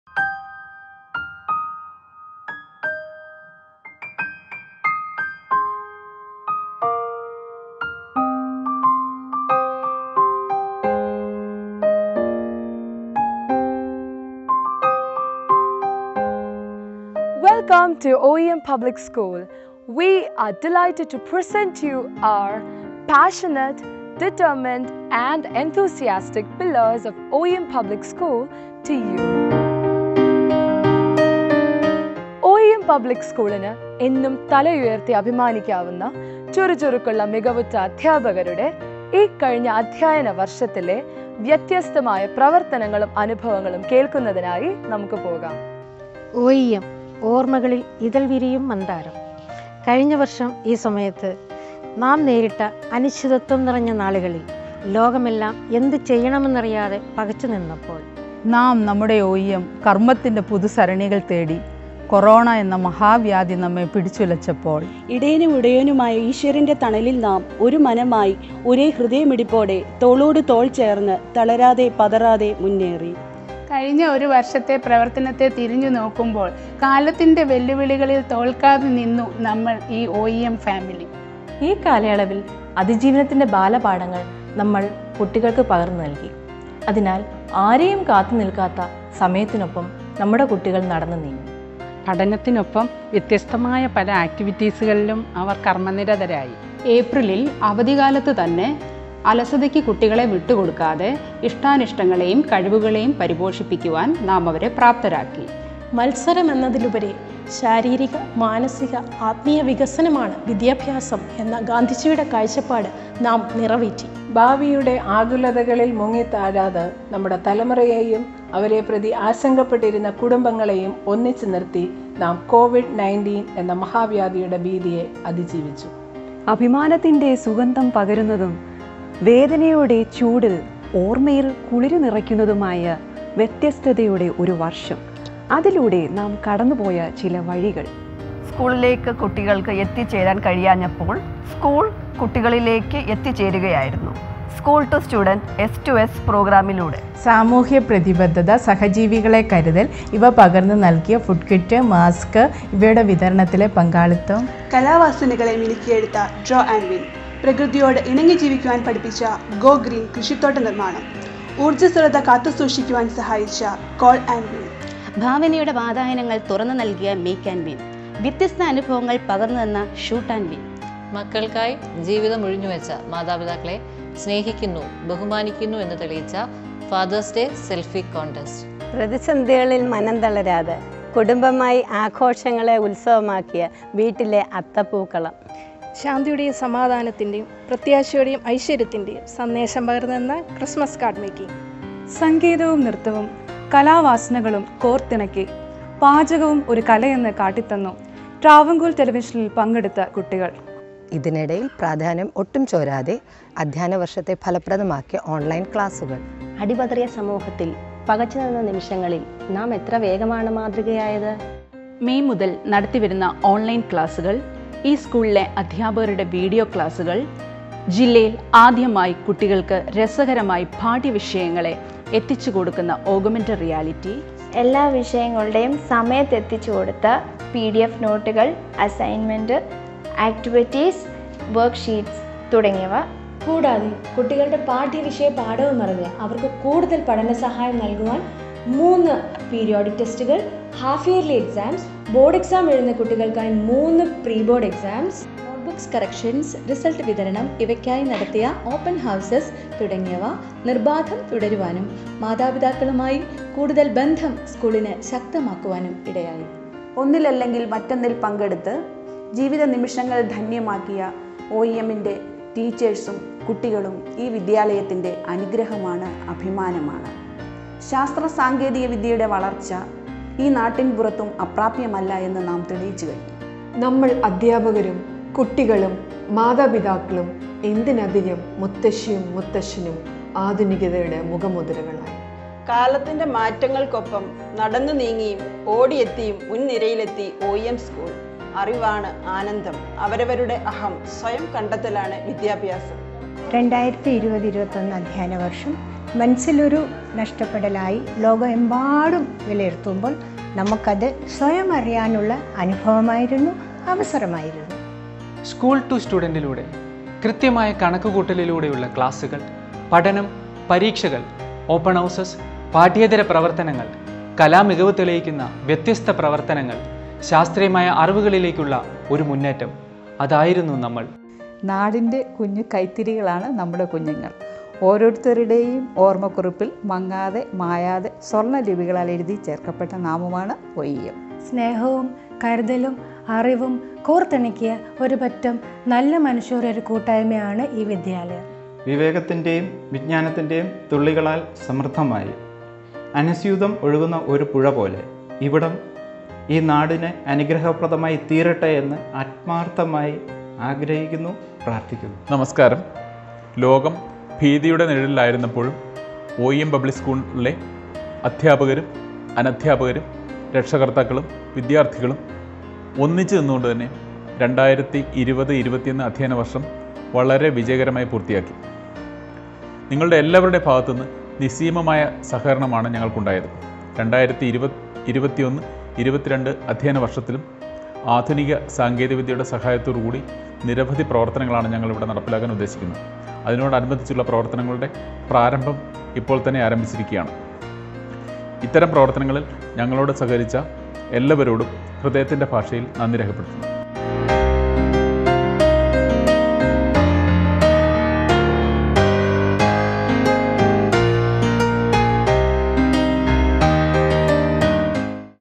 The strength of O.E.M PUBLIC SCHOOL| Words from our Faculty members
The priceless effort put forward by our faculties in pandemic situation is expressed in their own words.